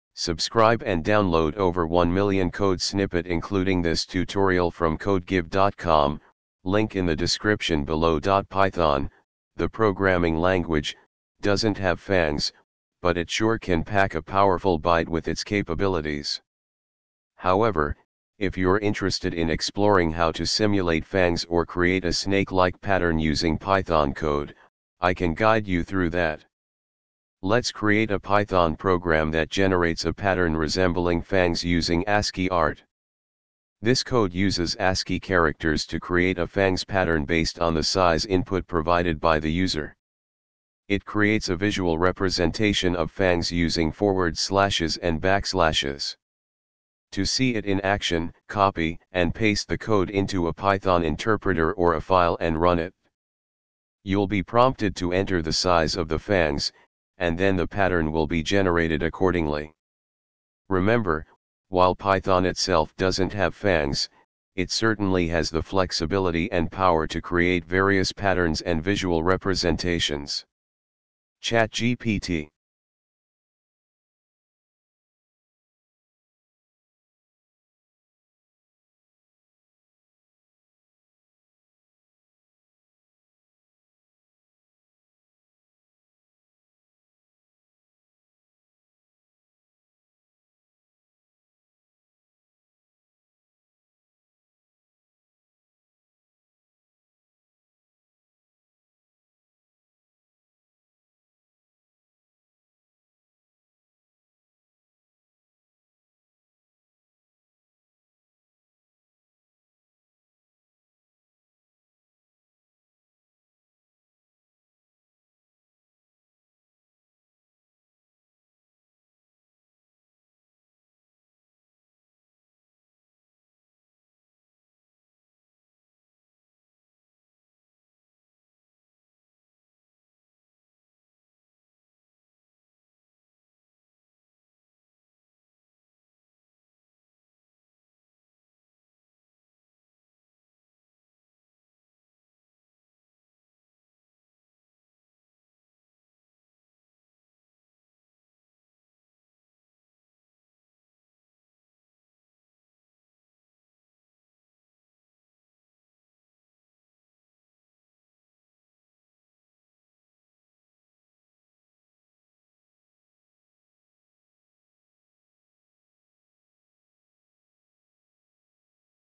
does a python have fangs
Python, the programming language, doesn’t have fangs, but it sure can pack a powerful bite with its capabilities! However, if you're interested in exploring how to simulate fangs or create a snake-like pattern using Python code, I can guide you through that. Let's create a Python program that generates a pattern resembling fangs using ASCII art.
This code uses ASCII characters to create a fangs pattern based on the size input provided by the user. It creates a visual representation of fangs using forward slashes and backslashes. To see it in action, copy and paste the code into a Python interpreter or a file and run it. You'll be prompted to enter the size of the fangs, and then the pattern will be generated accordingly.
Remember, while Python itself doesn't have fangs, it certainly has the flexibility and power to create various patterns and visual representations!
ChatGPT